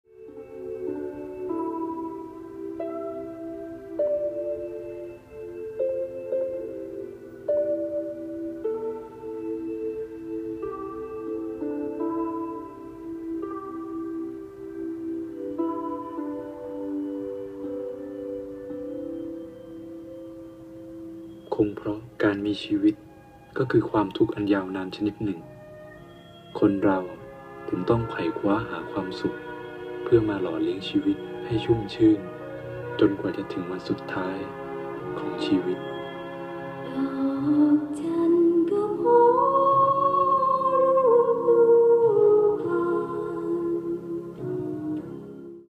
Finding the Black Tiger
Paying homage to Wisit Sasanatieng and Seua Dum on a 9 hours train ride across the Thai countryside. Shot on the iPhone5S.